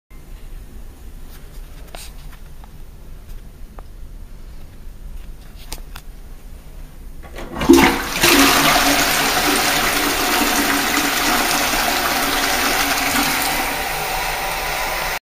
Caroma -Montague Rd
This Is at the Library i go to frequently.. ive filmed the disabled before
the Bowl is a caroma (Logo In Bowl) The Cistern is also a caroma
With a Very Good Flush